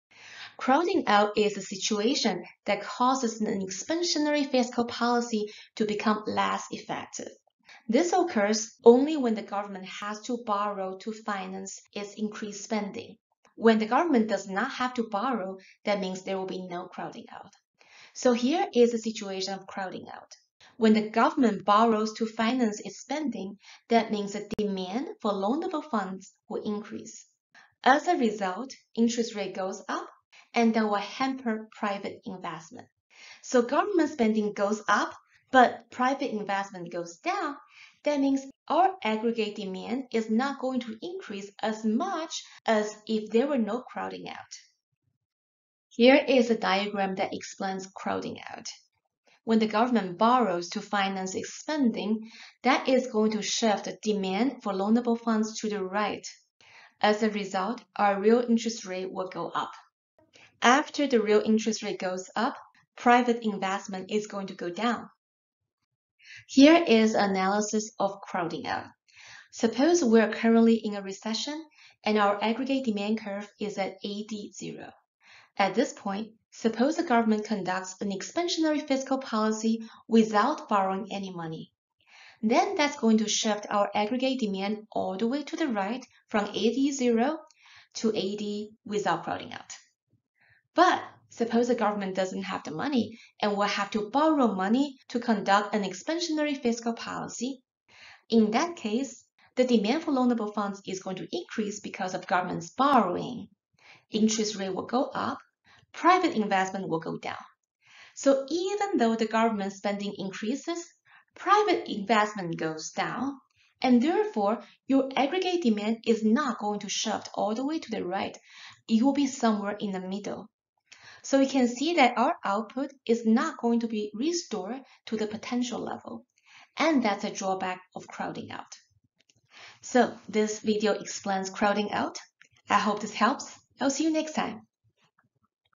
Fiscal Policy (3): Crowding Out
This clip explains what crowding out is and when it occurs-- essentially, when the government has to borrow to finance its spending, AD will not shift as much as if the government does not have to borrow.